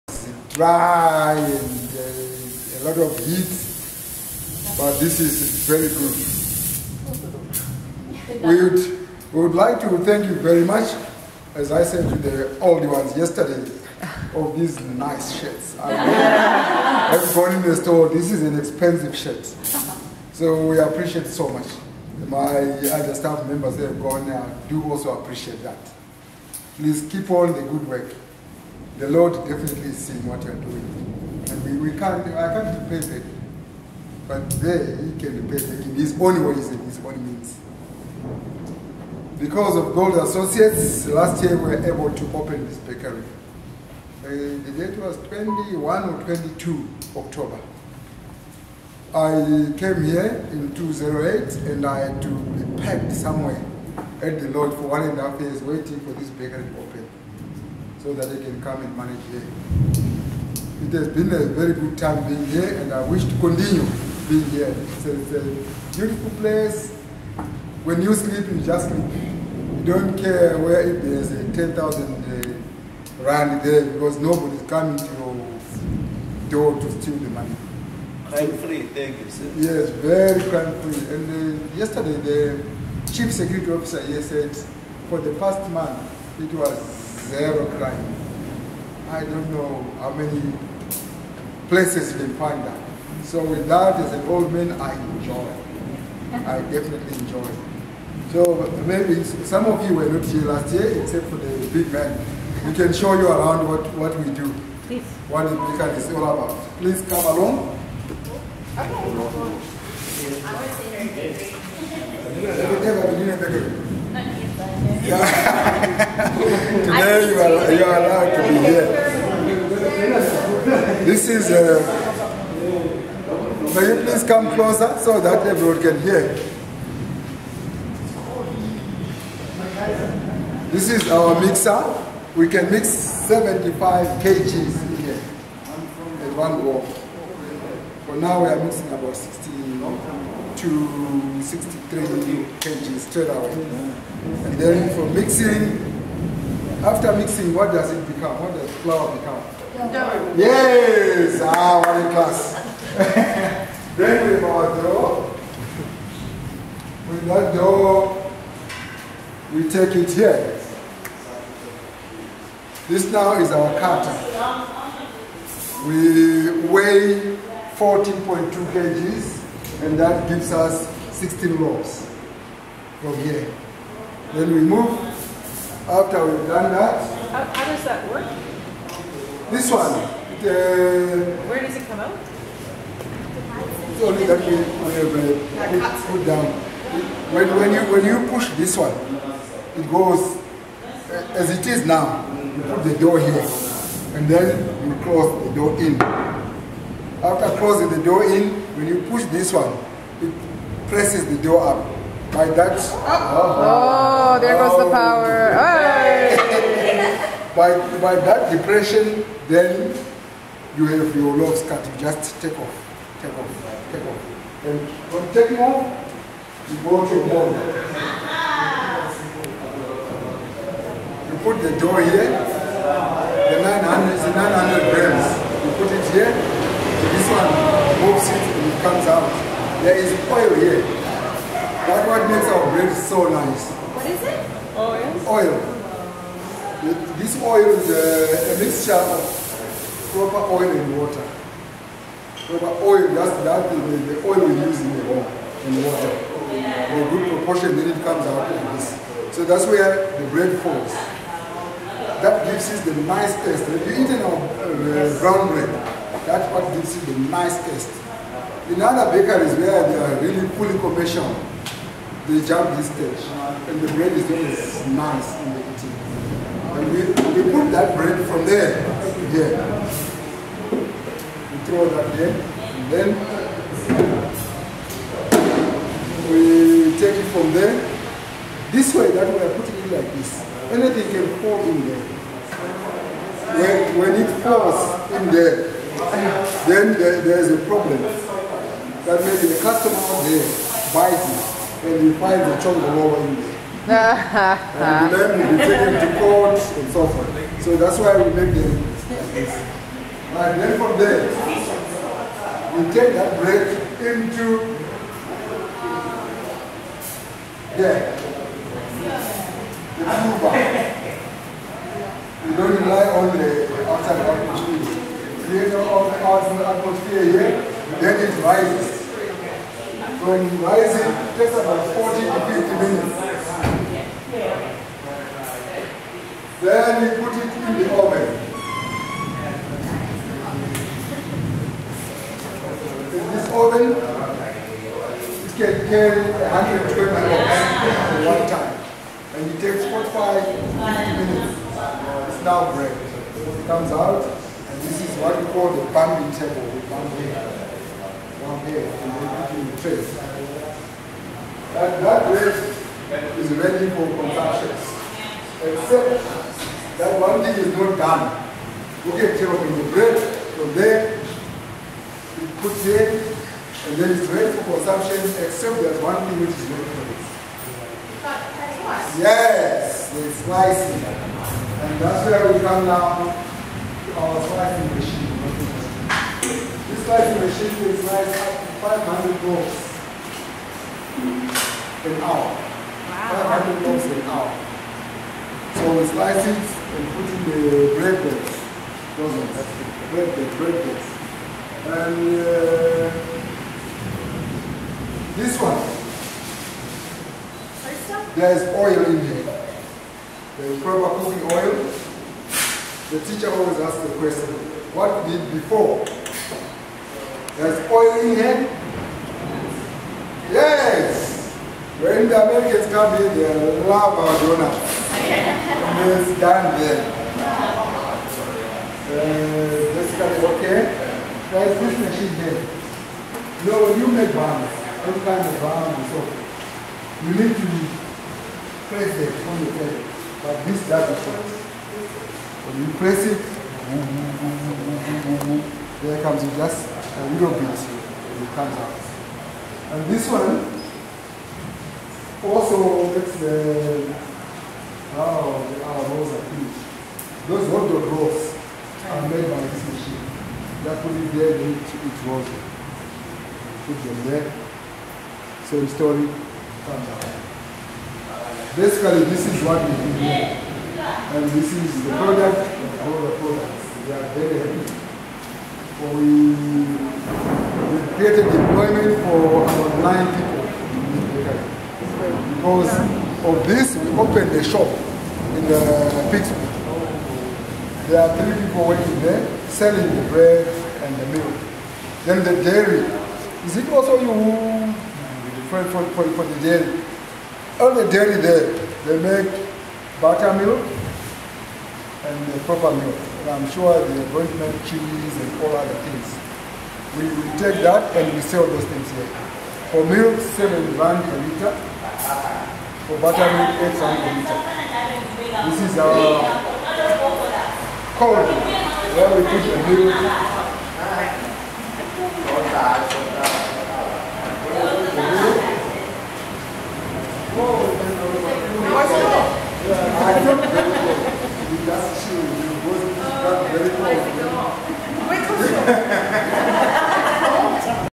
A Big Thank You to Golder from the Bulembu Bakery!
Tour of Bulembu bakery. Gentlemen thanking Golder for the business shirts they have donated. Explaining the reduction in crime rate from the opening of the bakery and how it has provided employment.